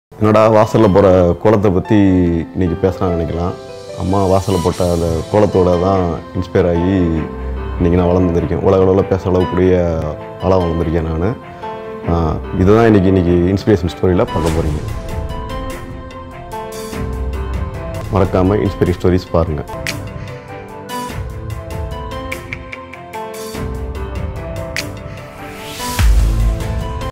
Inspiring Stories: Traditional Kolam as a Font UdhayaSankar Shares What Inspired this Creation Promo
Inspiring Stories: Traditional Kolam as a Font, Udhaya Sankar Shares What Inspired this Creation

Udhaya Sankar Draws Inspiration from the Traditional Kolam and Creates a  Font

Category Entertainment